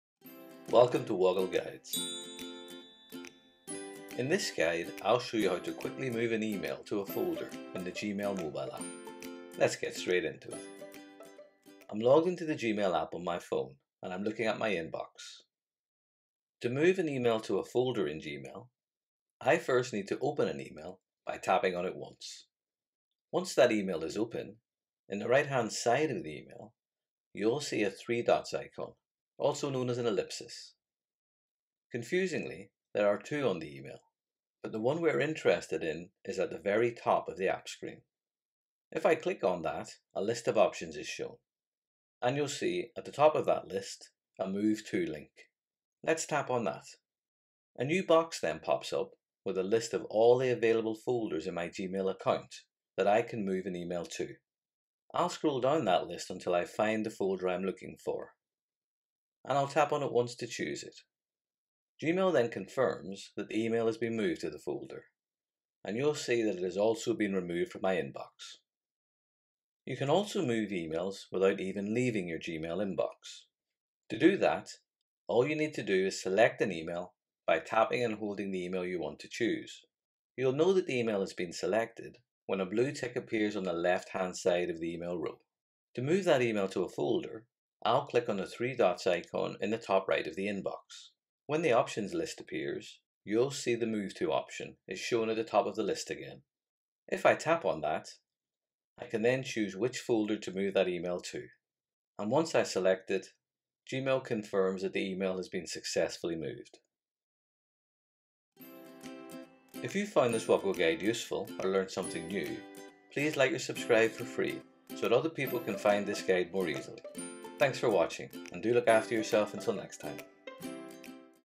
How to move an email to a folder in the Gmail mobile app
In this guide, I’ll explain how to move an email to a folder in the Gmail mobile app

🕔  Key Moments
00:00 | Introduction
00:11 | How to move an email to a folder in the Gmail mobile app
01:48 | Wrap up